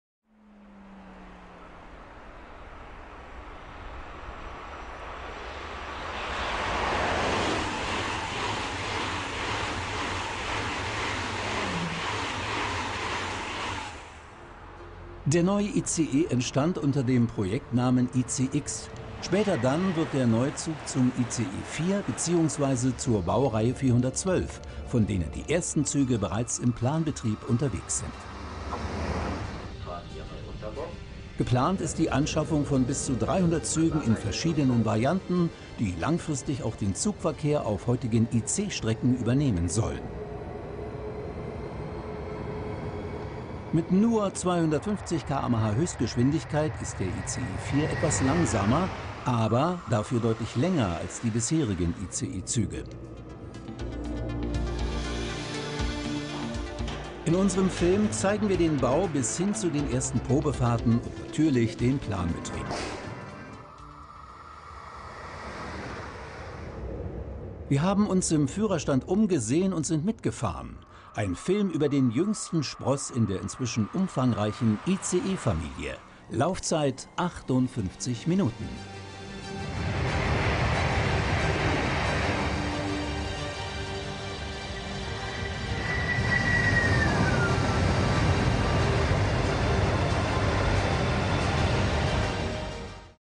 ICE 4 oder Baureihe 412
Der neue ICE entstand unter dem Projektnamen ICx. Später dann wird der neue Zug zum ICE 4 bzw. zur Baureihe 412, von denen die ersten Züge inzwischen im Regelbetrieb unterwegs sind. Geplant ist die Anschaffung von bis zu 300 Zügen in verschiedenen Varianten, die langfristig auch den Zugverkehr auf heutigen IC-Strecken übernehmen sollen. Mit „nur“ 250 km/h Höchstgeschwindigkeit ist der ICE 4 etwas langsamer, aber dafür deutlich länger als die bisherigen ICE-Züge. In unserem Film zeigen wir den Bau bis hin zu den ersten Probefahrten und natürlich den Planbetrieb. Wir haben uns im Führerstand umgesehen und sind mitgefahren. Ein Film über den jüngsten Spross in der inzwischen umfangreichen ICE-Familie. Laufzeit 58 Minuten. Hier mehr
Für nur 2,99/Monat Kanalmitglied werden und exklusive Vorteile erhalten: